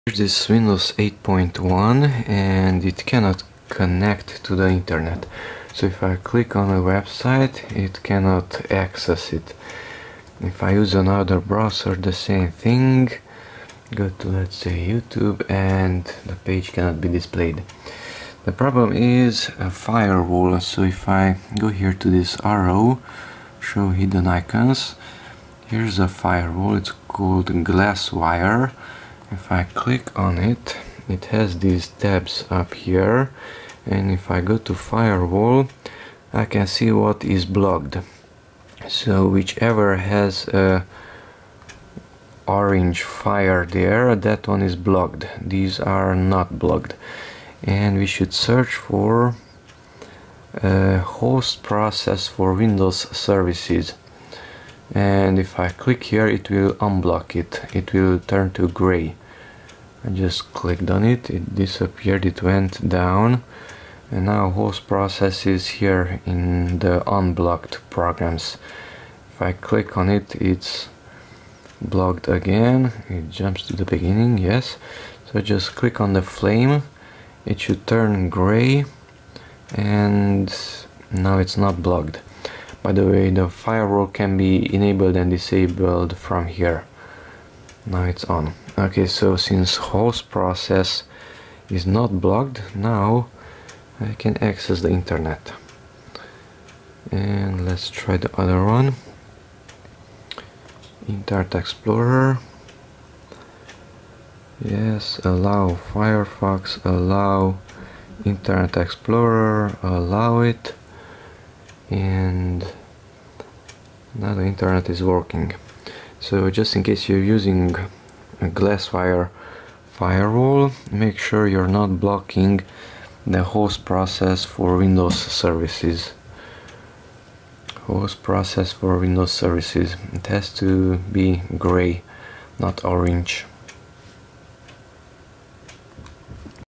Can't connect to internet (GlassWire firewall, Host process for windows services)
If you accidentally blocked "Host process for windows services", then unblock it.

Firefox message: "Hmm. We're having trouble finding that site."
Internet Explorer message: "This page can't be displayed"